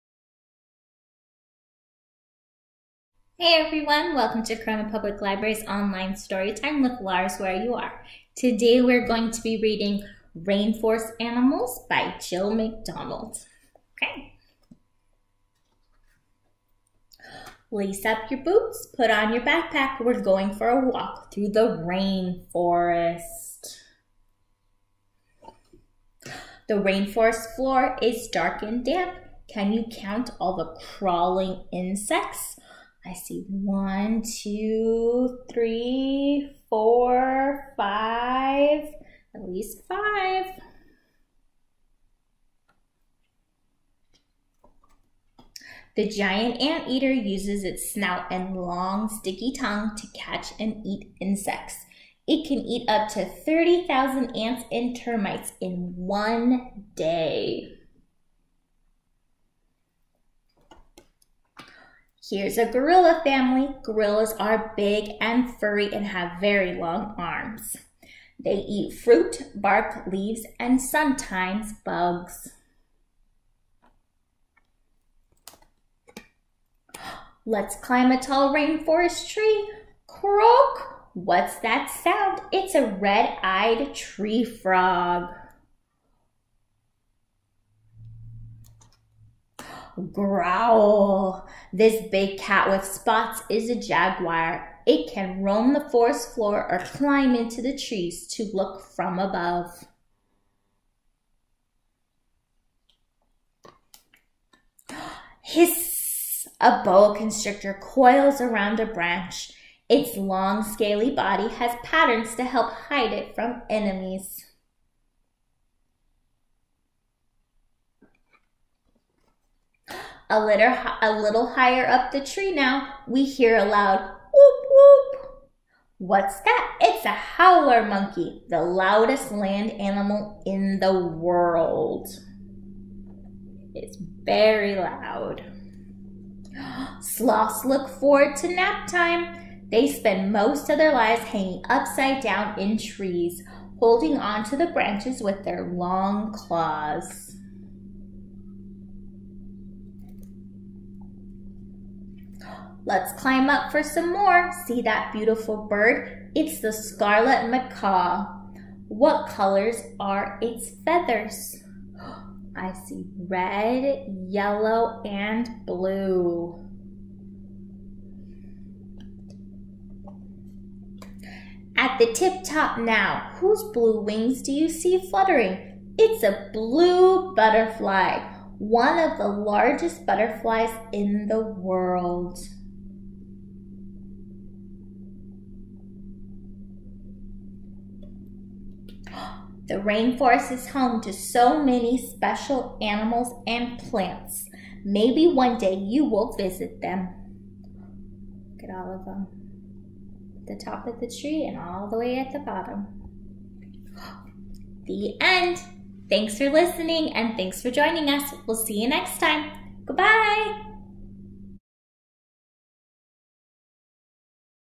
Storytime with Ms. Jennifer - Hello World!
Join the Corona Public Library's Storytellers for stories, rhymes & songs!
Remember to practice your early literacy skills at home: read, talk, sing, play, and write!

Story: Hello World! Rainforest Animals by Jill McDonald